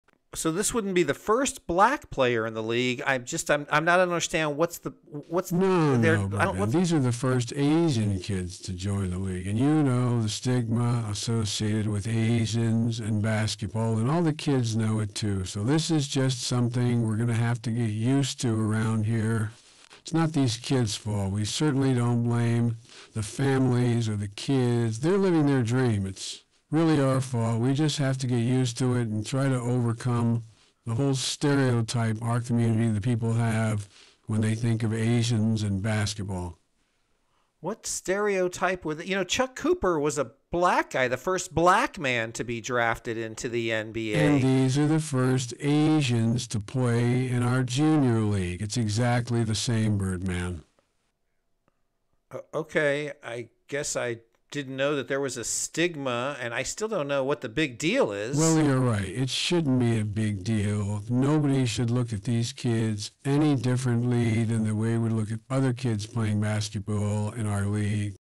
Asians Dominate the NBA?
In a small community in America today. many of the same challenges persist with discrimination. We will talk with a member of one who confirms it for us. But who is being stereotyped?